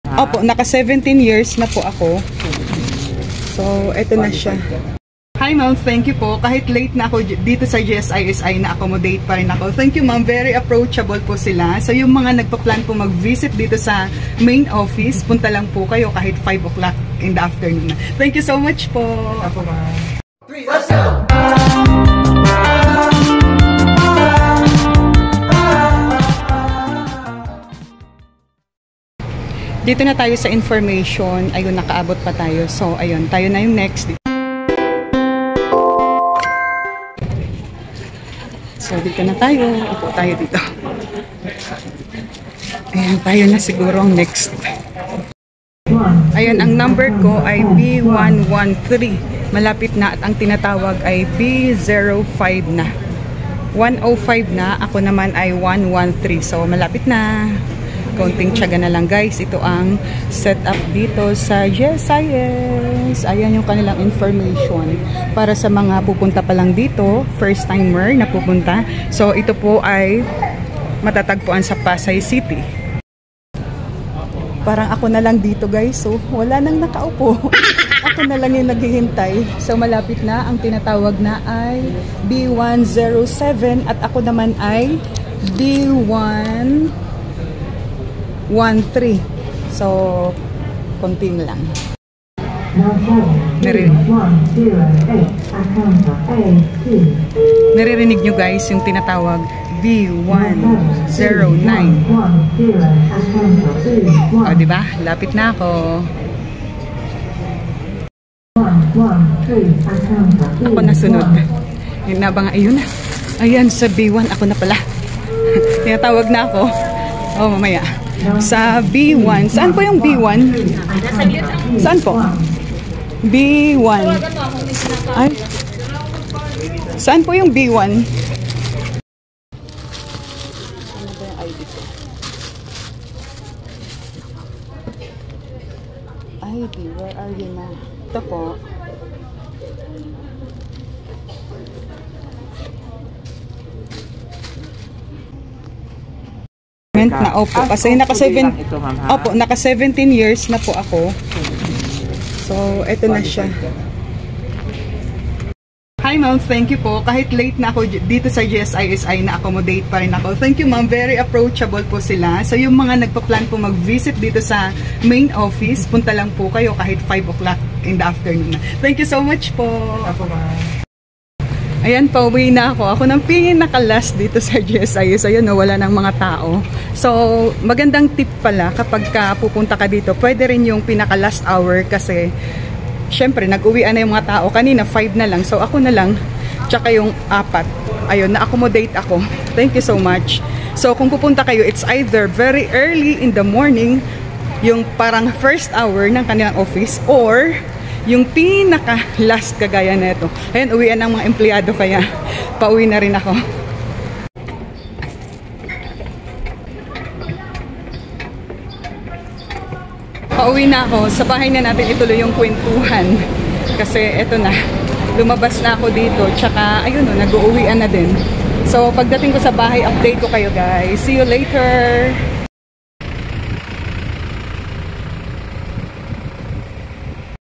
BEST TIME PARA PUMUNTA SA GSIS MAIN OFFICE NA DI NA MAGHINTAY NG PILA I Nice Day Vlog
Sa video na ito share ko po ang isang tip para makatulong kelan ang best time na pumunta sa GSIS Main Office Pasay City.

Para sa mga di pa nakapanood ng ating mga episode ng " GSIStorya " ito po ang mga link mapapanood nyo ang mga Episode sa mga link na to.👇👇

Happy Watching and ...